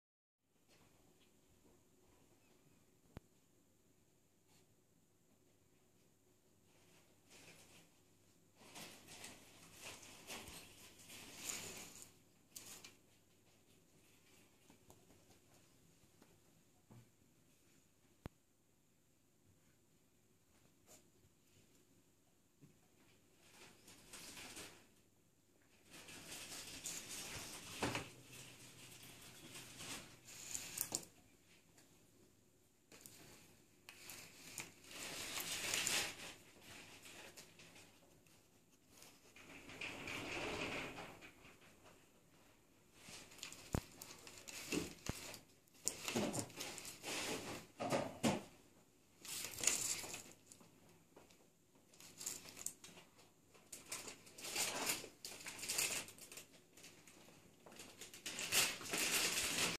Yoshi tout foufou
L'herbe à chats, un sacré stimulant